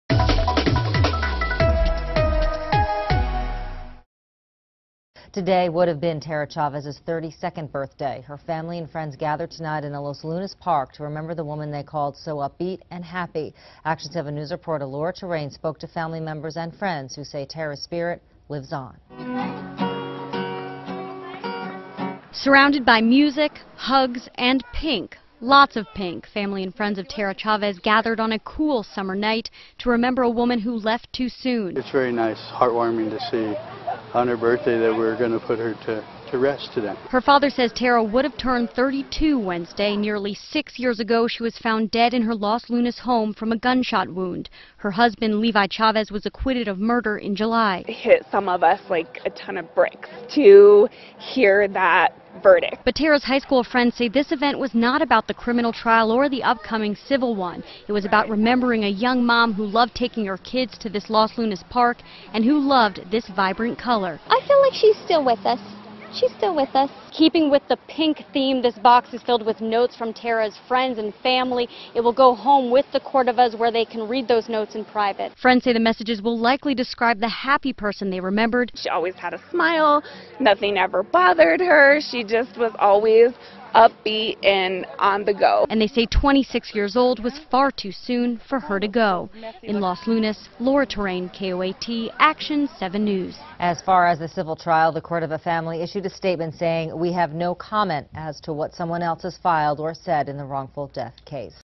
Family, Friends gather to remember Tera Chavez
Tera Chavez would have been 32 on Wednesday, family and friends gathered in a Los Lunas park to remember her.